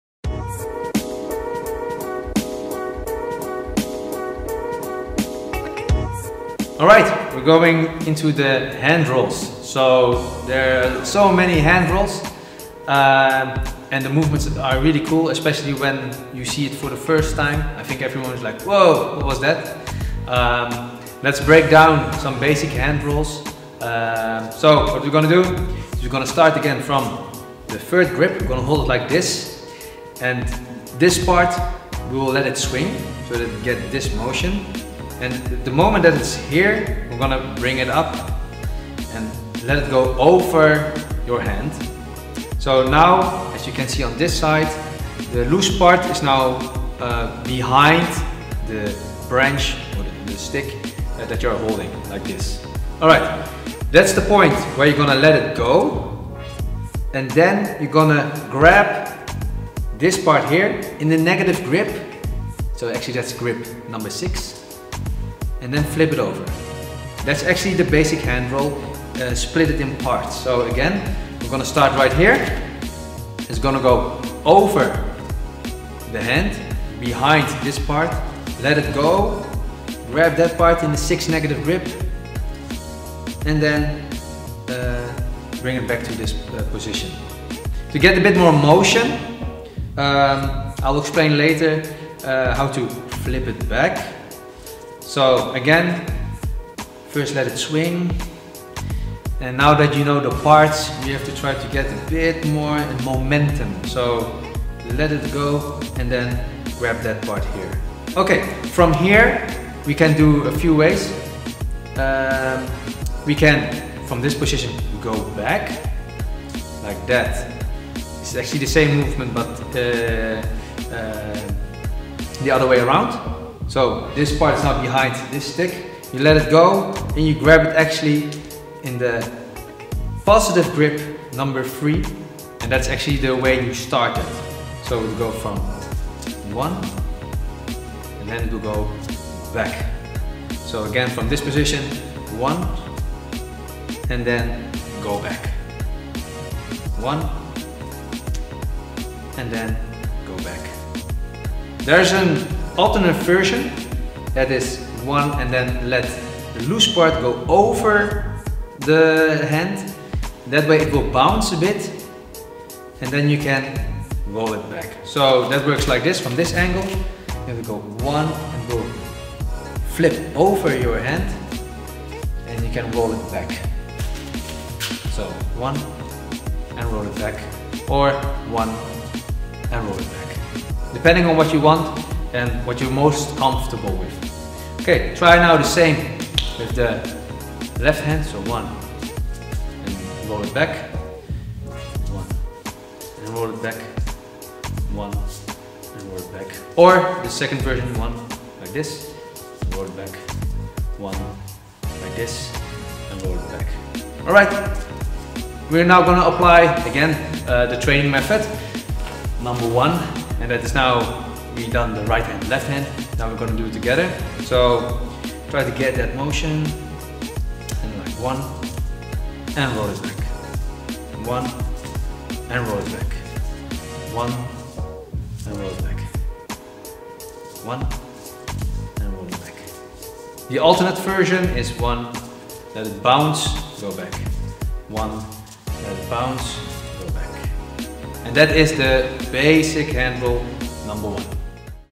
Nunchaku freestyle tutorial 8 | Handroll 1 | LVL: Beginners | Joris vd Berg
Nunchaku tutorial 8 beginners - Handroll 1
In this tutorial I  will explain handroll number 1.

There are many possibilities with the handrolls. this is the first basic handroll.